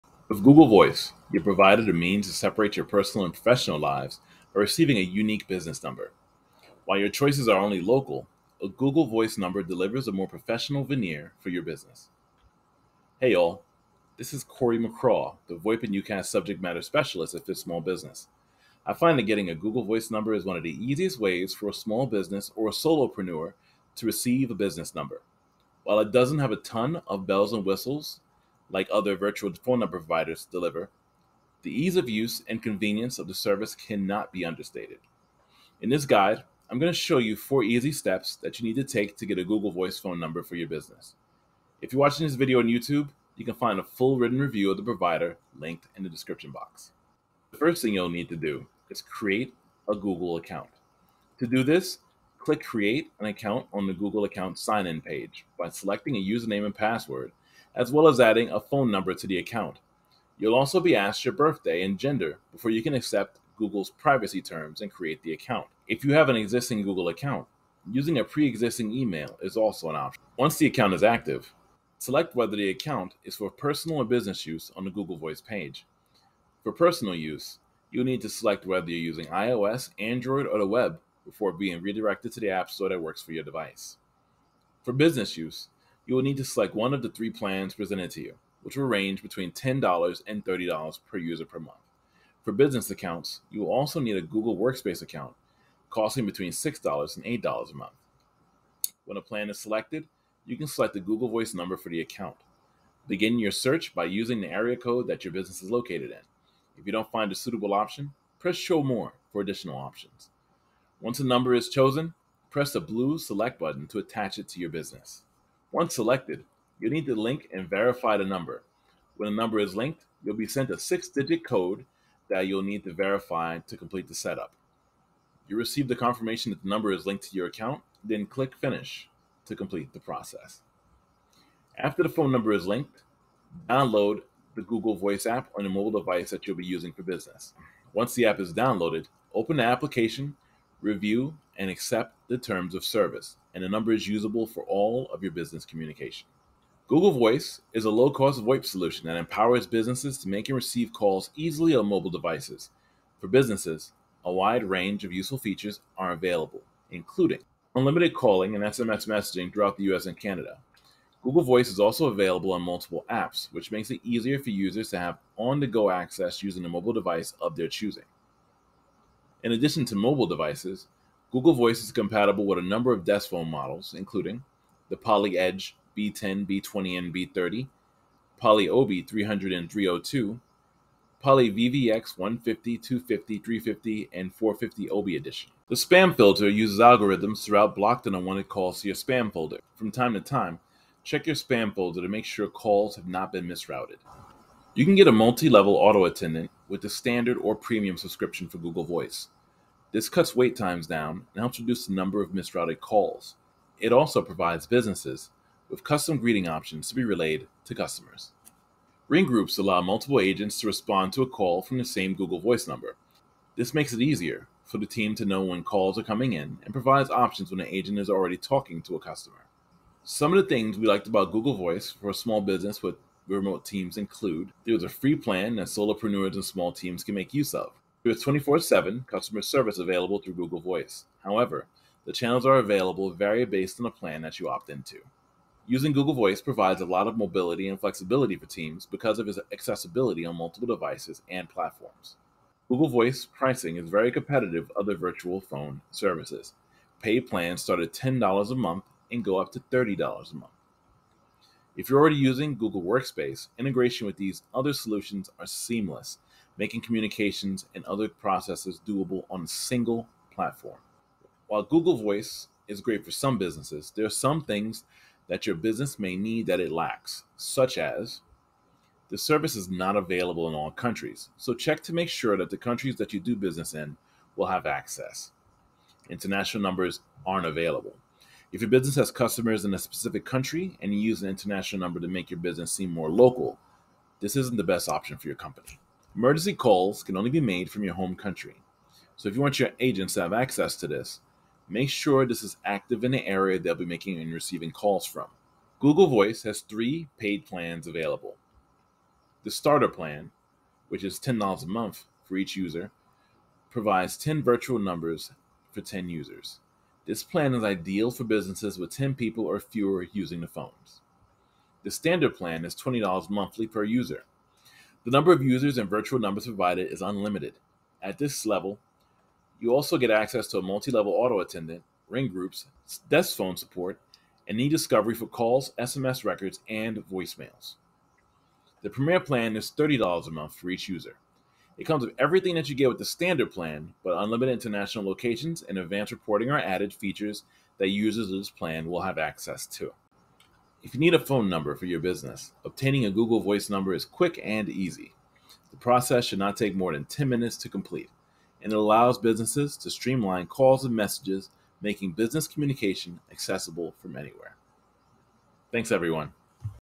How to Get a Google Voice Number for Your Small Businesses
Watch this quick video on how you can get a Google Voice number for free to help separate your work and personal calls.

Chapters:
0:00 Introduction
0:50 Step 1: Create a Google Account
1:12 Step 2: Google Voice Number Selection
1:59 Step 3: Link & Verify Your Existing Number
2:14 Download Google Voice App
2:29 Google Voice Features
3:40 Google Voice Pros and Cons
4:48 Plans and Pricing
5:45 Bottom Line